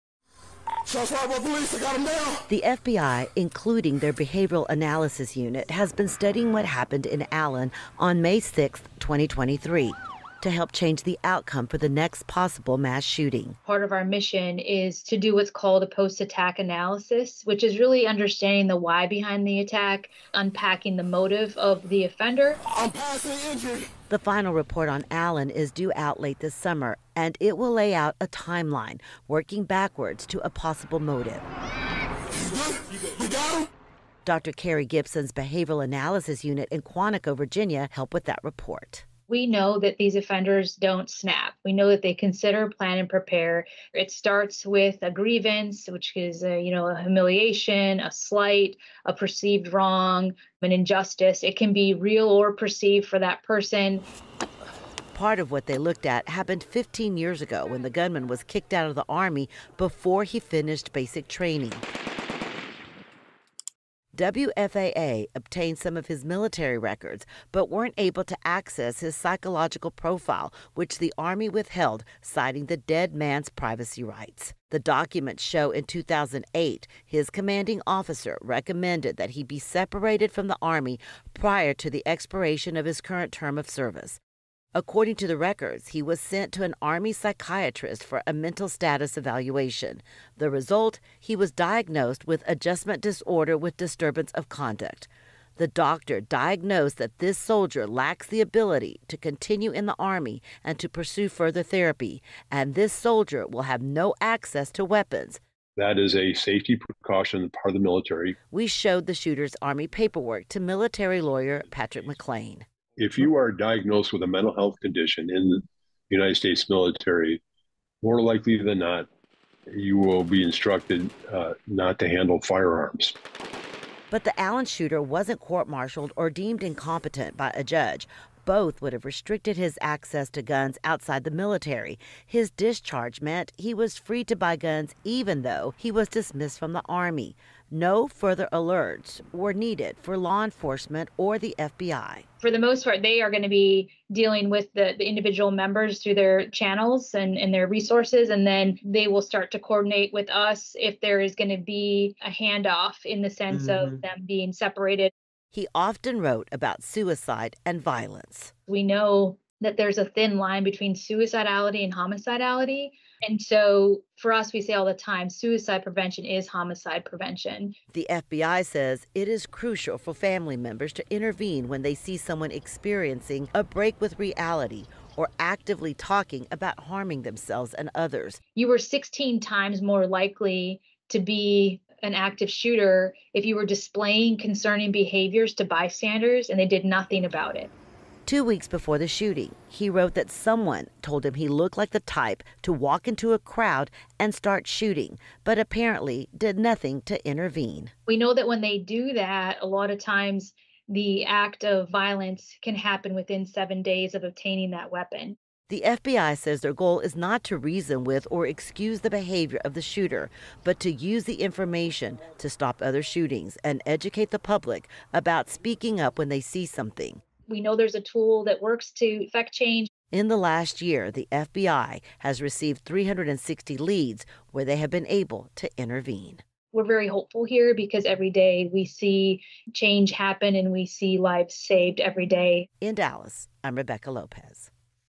Over 1 year after Allen mall shooting, FBI continues to study what happened
The FBI continues to figure what happened and how similar situations can be avoided in the future.